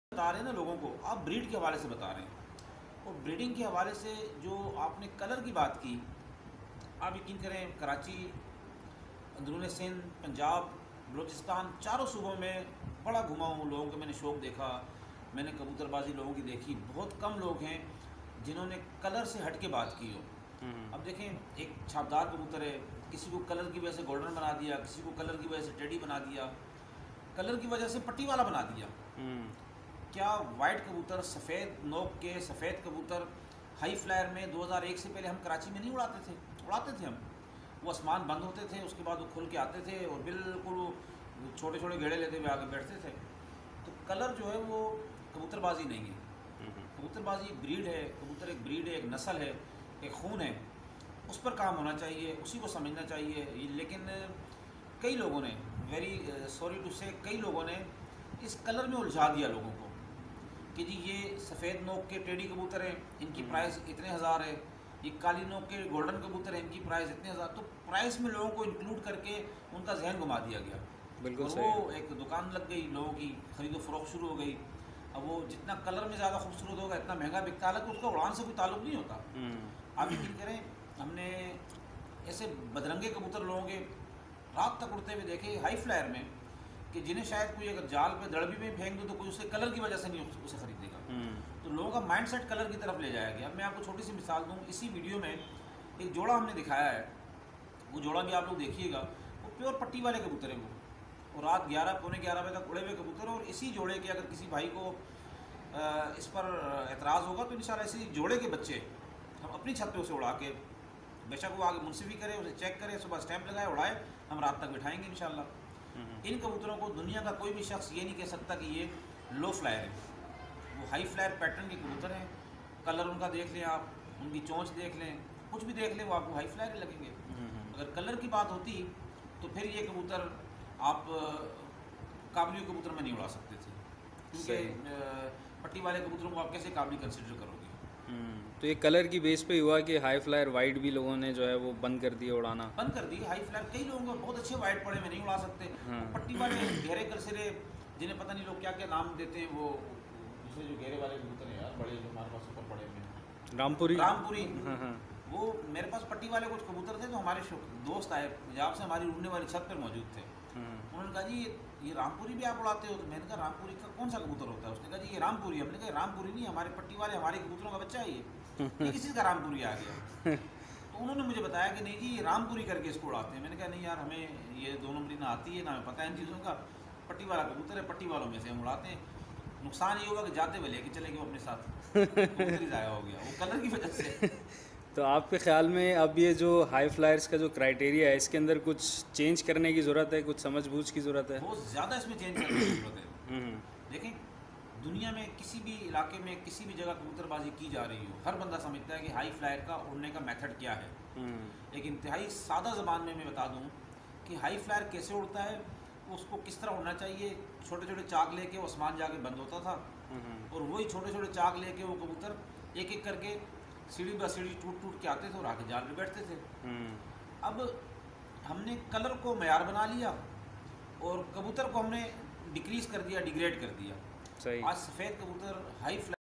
Ustad Abdul Basit says Kabootar/Pigeon Color/Rang Breed/Nasal/Bloodline ki pehchan nahi ! 😊
For Full Video follow the link below ;